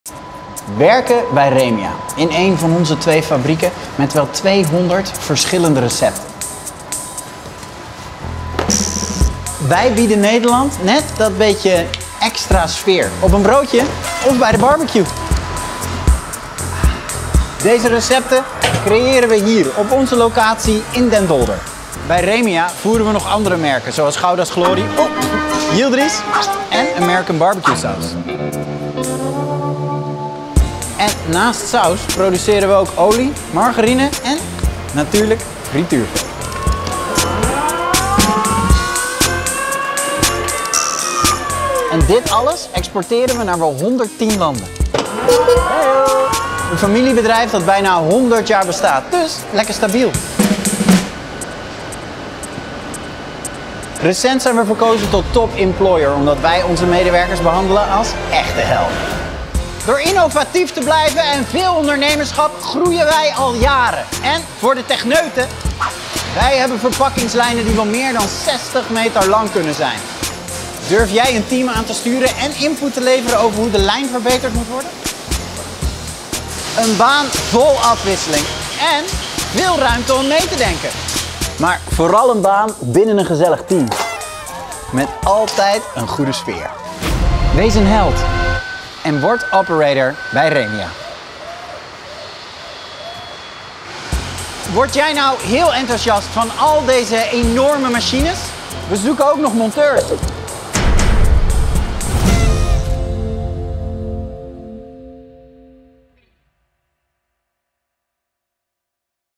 Werken bij Remia: Word jij een Remia Hero?
Van iedere maaltijd een feestje maken: dé missie van sauzenproducent Remia en iedereen die er werkt. We zijn op zoek naar nieuwe helden! Zin om aan de slag te gaan bij dit dynamische en gezellige familiebedrijf? Kom ook werken bij Remia! Kijk op Remia-heroes.nl